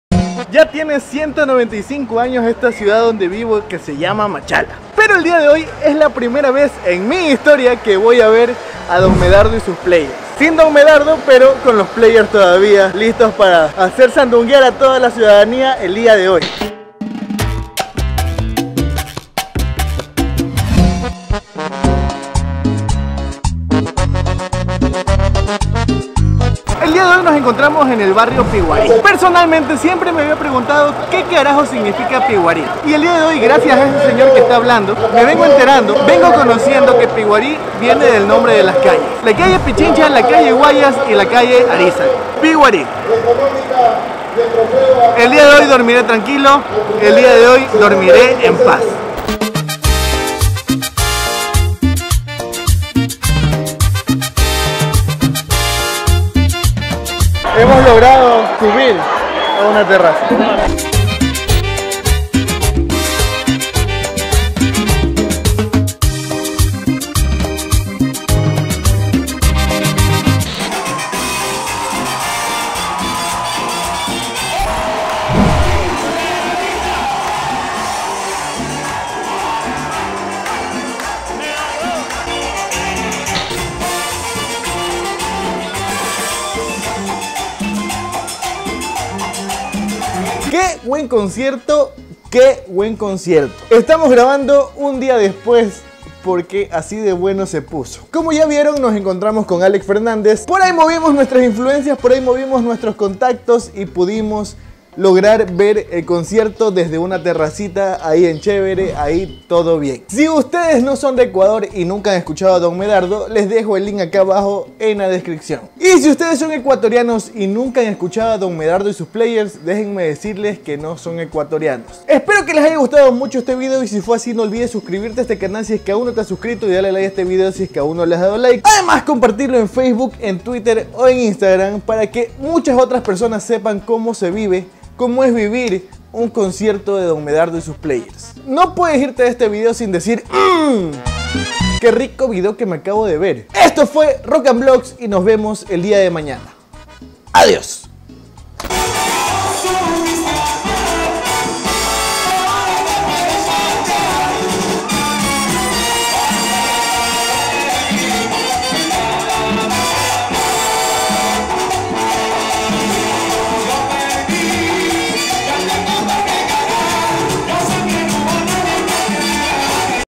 Así se vivió el concierto de Don Medardo y sus Players en Machala
Para ti Chonera linda!!!

¿Qué equipos uso?
Video: Canon T3i
Sonido: Rode Mic Lavalier
Iluminación: Dios Sol

Edición: Sony vegas Pro / Adobe Premiere Pro